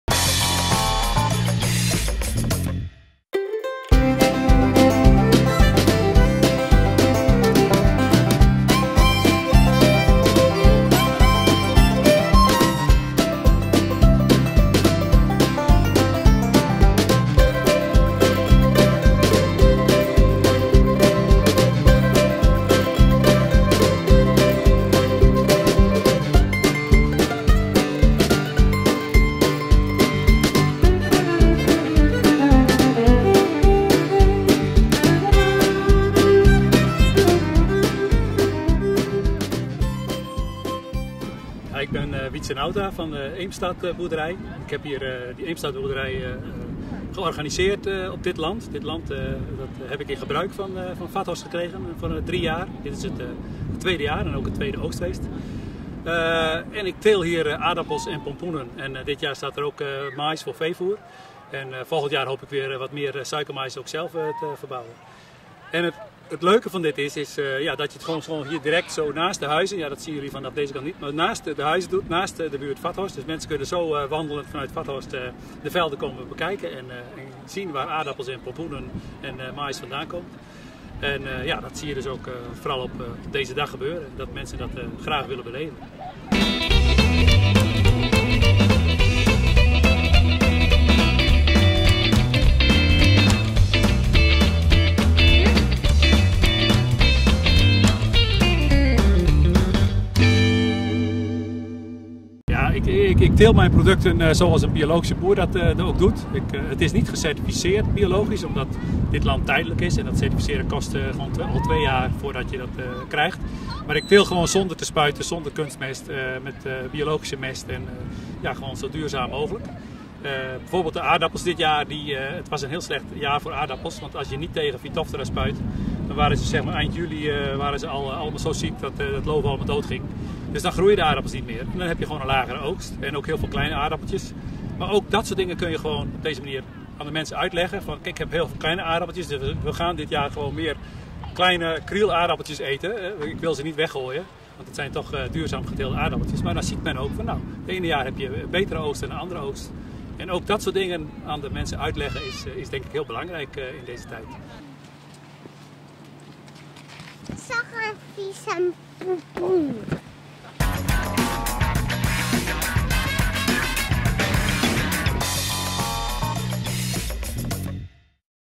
Oogstfeest 2014
Op zondag 21 september vond het Oogstfeest plaats in Vathorst. Het feest werd georganiseerd door EEMSTADboerderij.